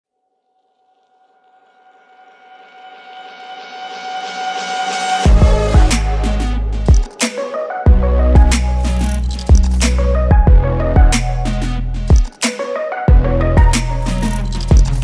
Nice walk and encounter the Beagle dogs on the beach in Thailand.
This two senior dogs are 7 and 14 years old.